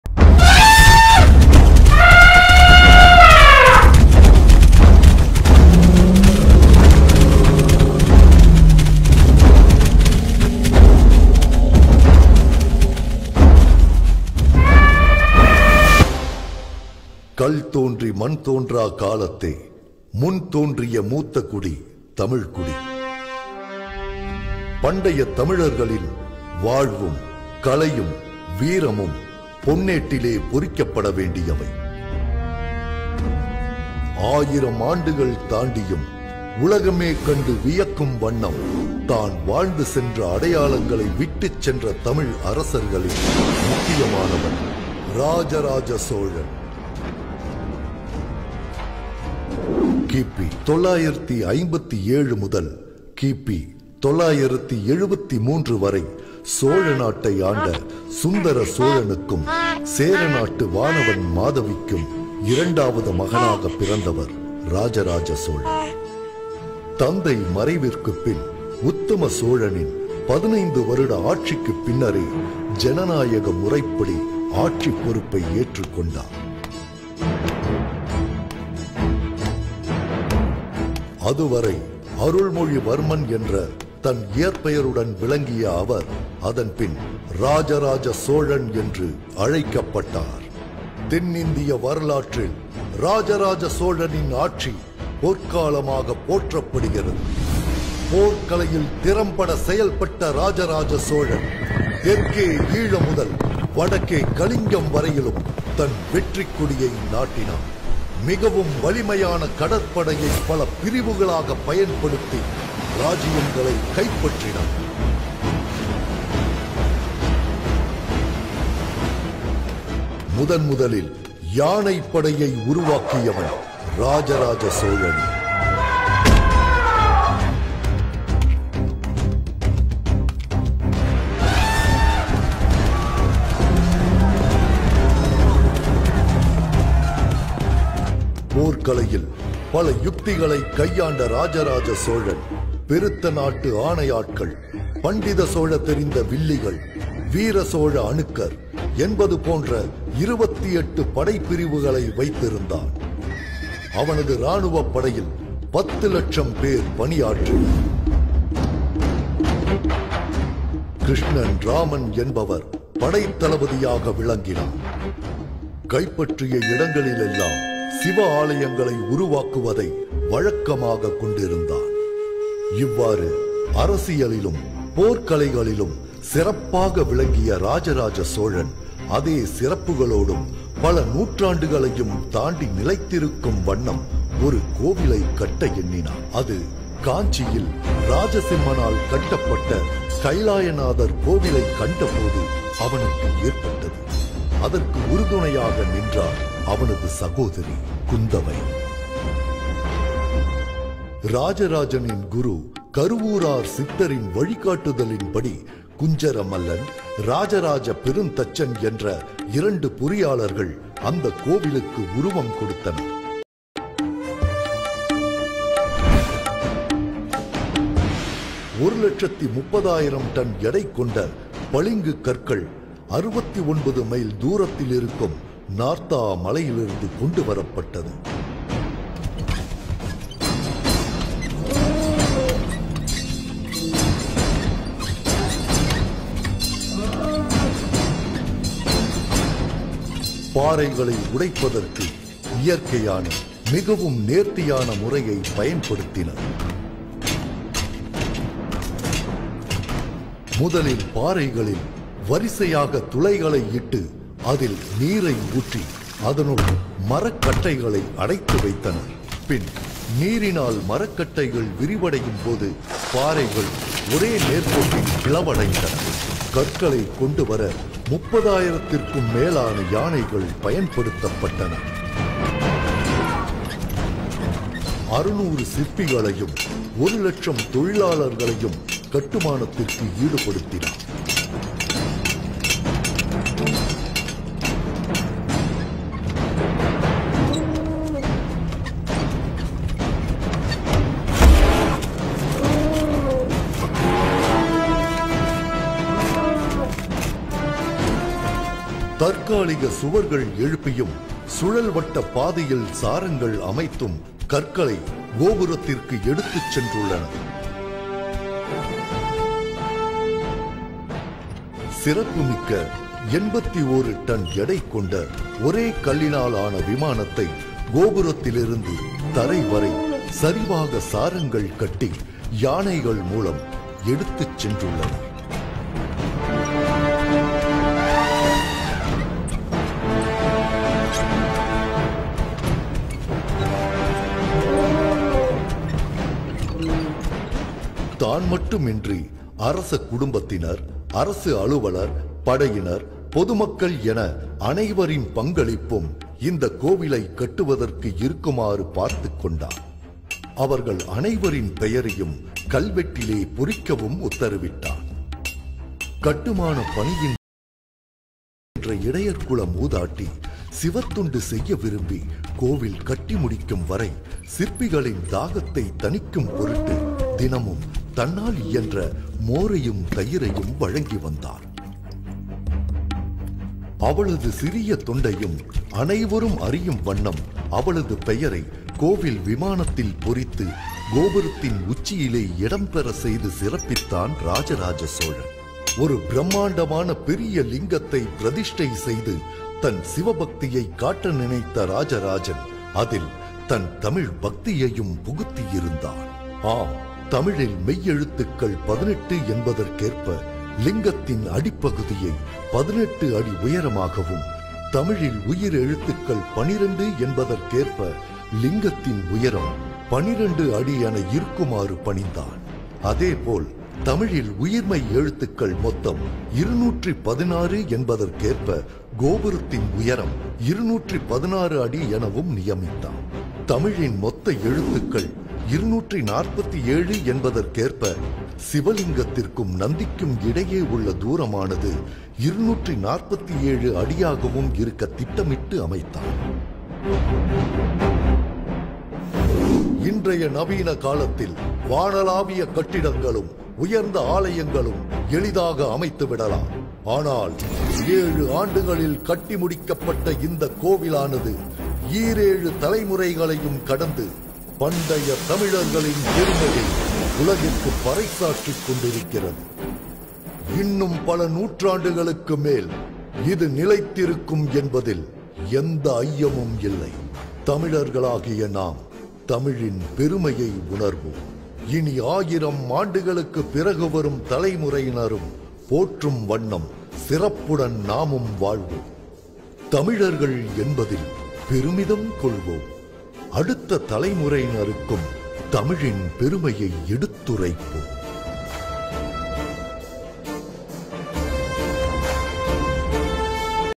Raja Raja Cholan Story in Tamil | Historical brihadeshwara temple | Thanjai Periya kovil
Tamil Thanjavur temple golden history